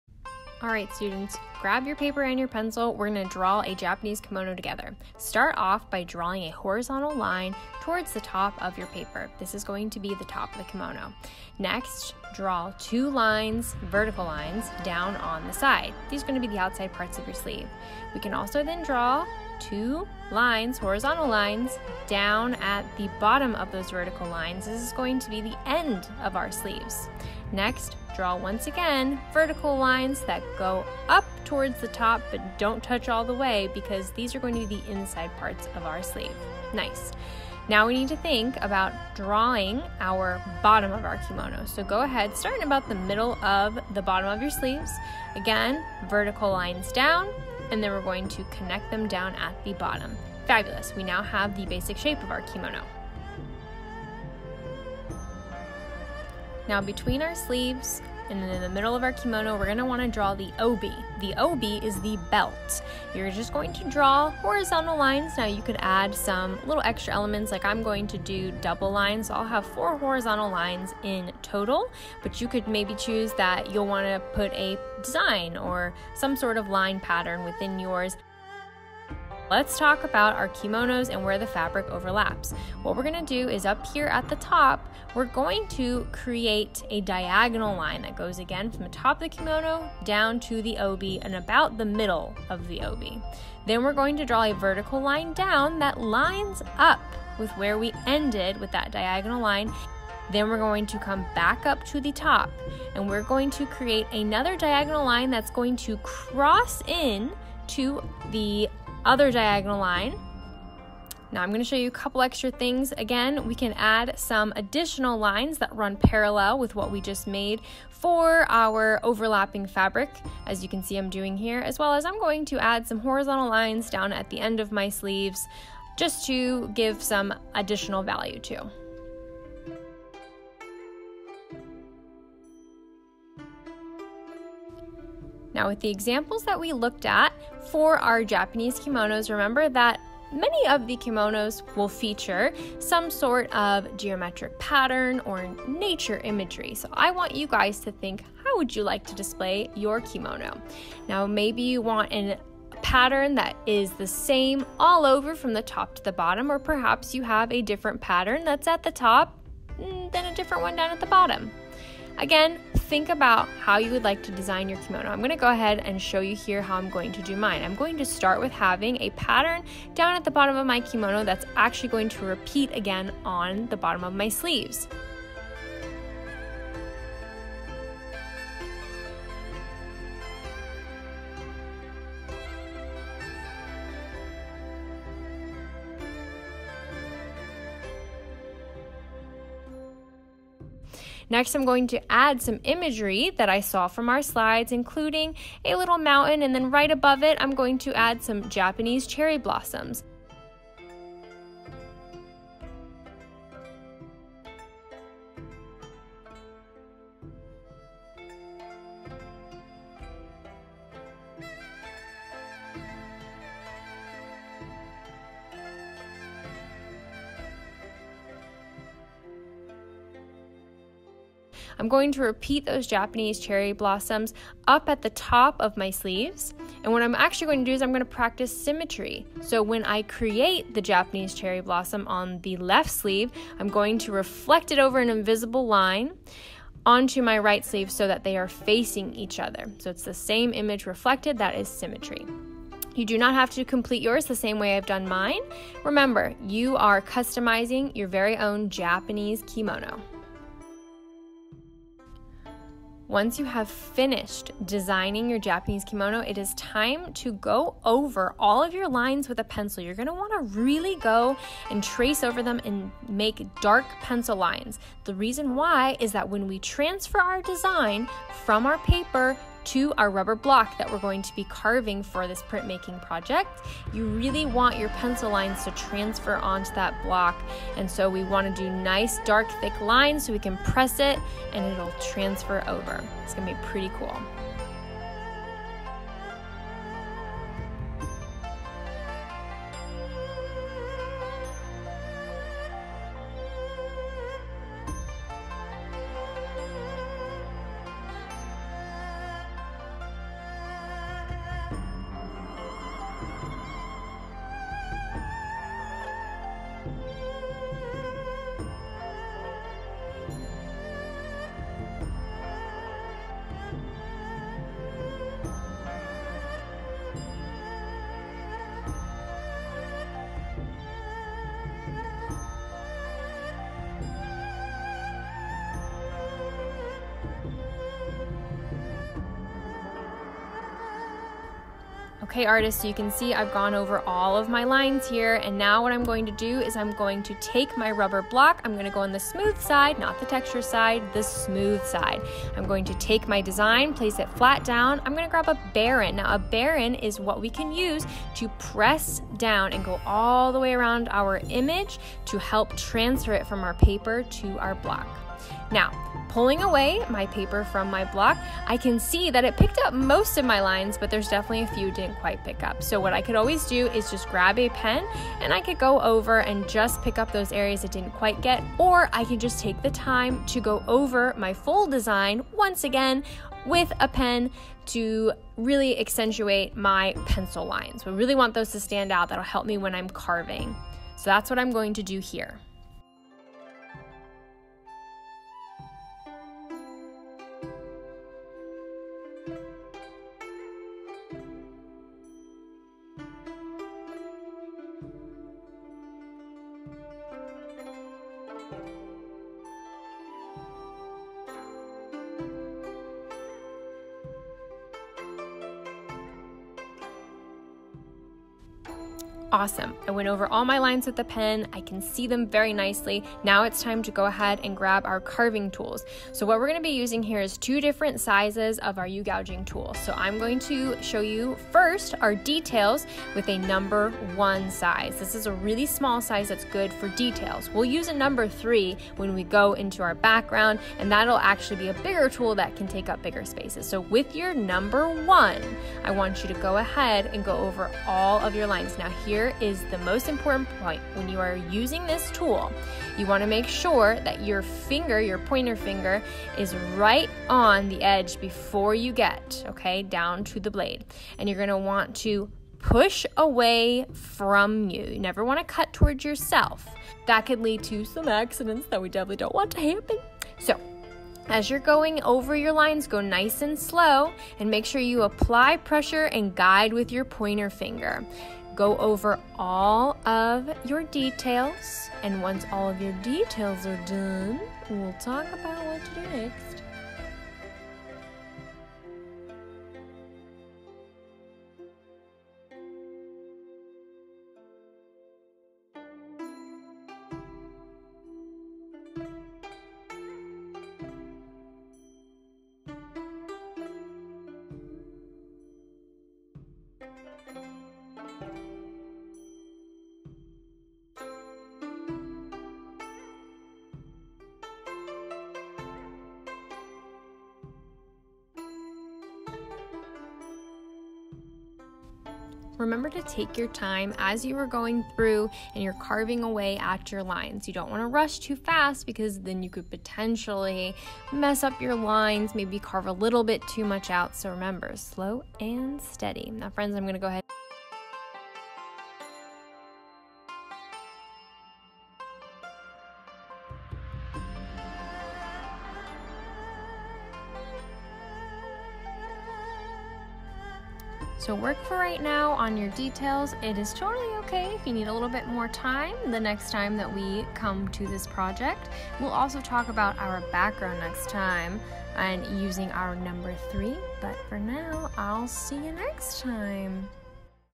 PRINTMAKING: Japanese Kimonos, pt 1
Howdy, artists! This week, we'll design a Japanese Kimono that we'll then transfer to our Rubber Block and begin carving into.